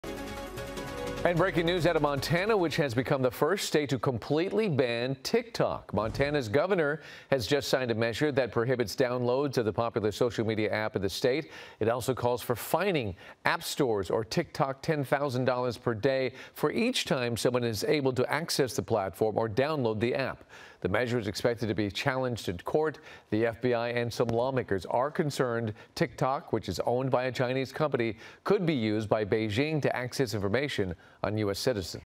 Montana becomes 1st state to ban TikTok
The measure is expected to be challenged legally and will serve as a testing ground for the TikTok-free America that many national lawmakers have envisioned. FULL STORY